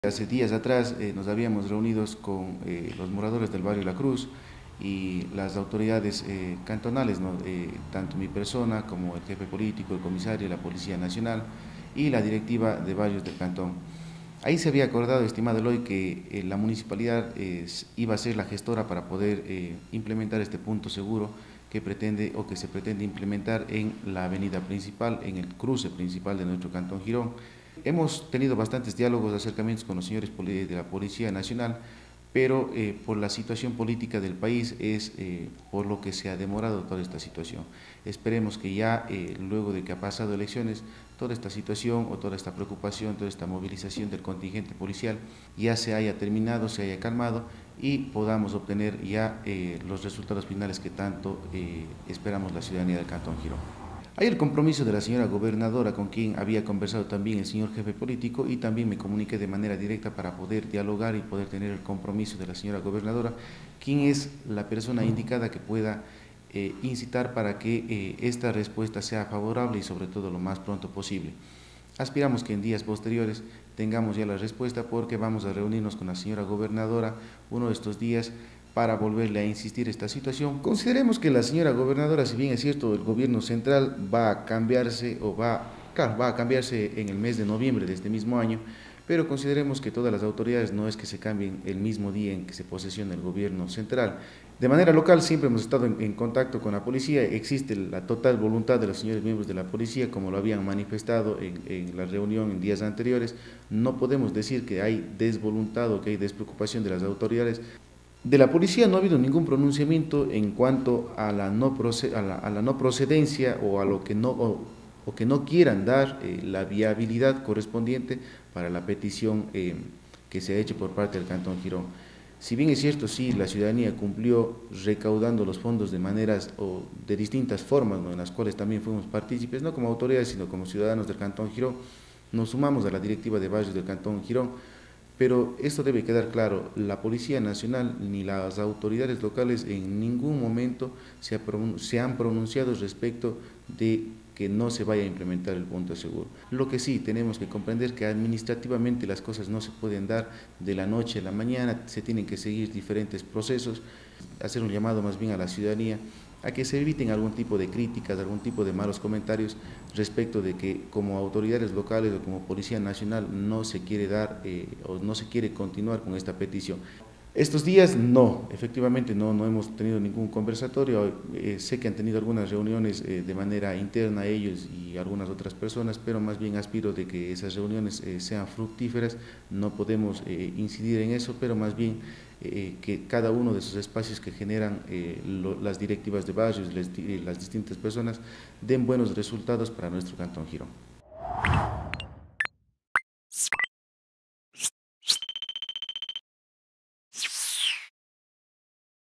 Alcalde de Girón habló sobre avances de la implementación de la UPC
Desde hace meses o quizá años, la dirigencia de los barrios de Girón lleva buscando implementar una UPC en el barrio la Cruz, han dialogado con los jefes de policía, autoridades municipales y, han cumplido exigencias; pero, la aspiración no se hace efectiva, hace unas semanas en una asamblea el alcalde de Girón se comprometió en tomar a su cargo la gestión. En una entrevista, la autoridad municipal habló sobre los avances.